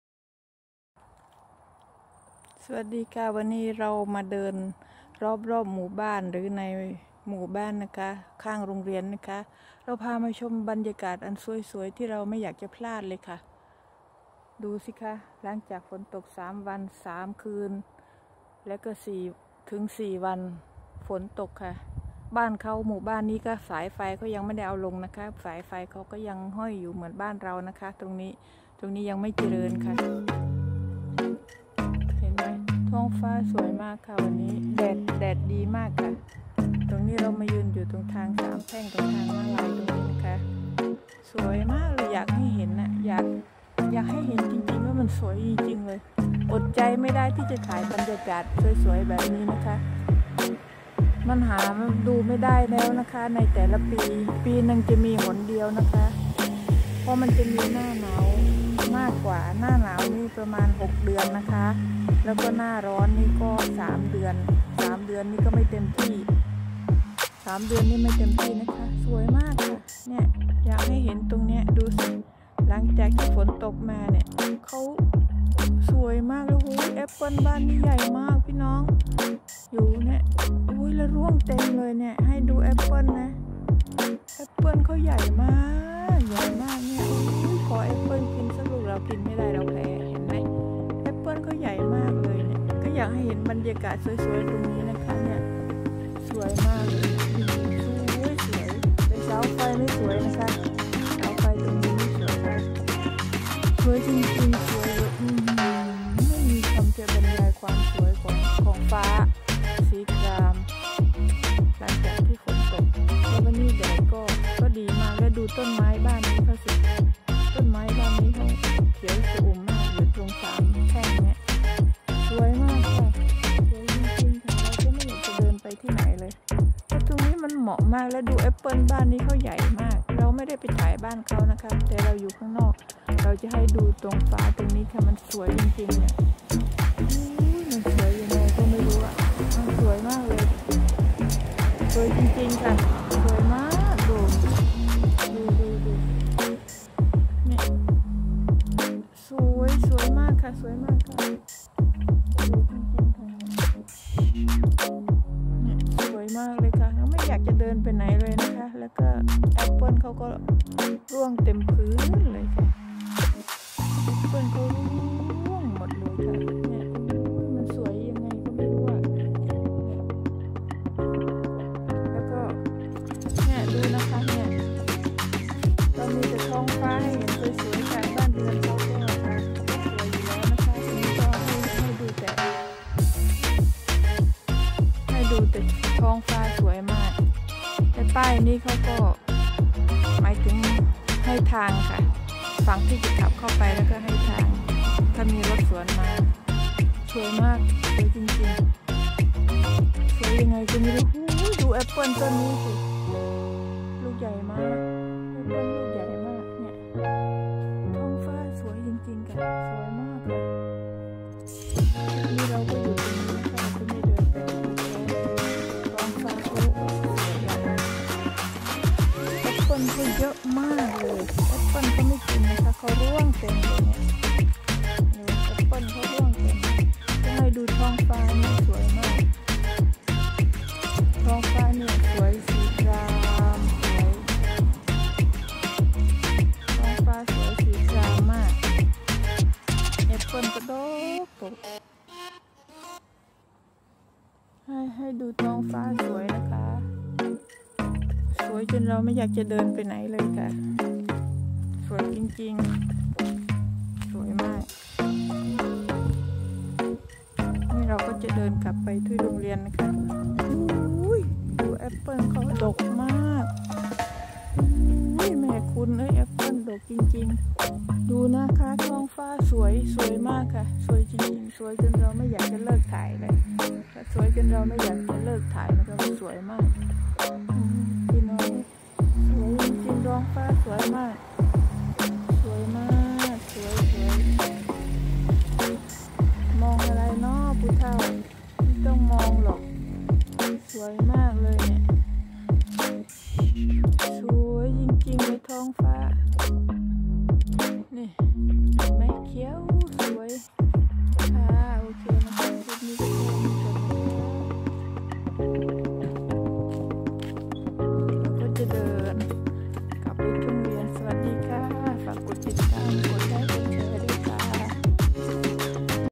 พามาเดินรวบๆๆ#ในๆหมู่บัาน# Apple
พาเดินชมรอบๆและในหมู่บ้านและก็เห็นลูกแอปเปิ้ลสวยมากลูกใหญ่มาก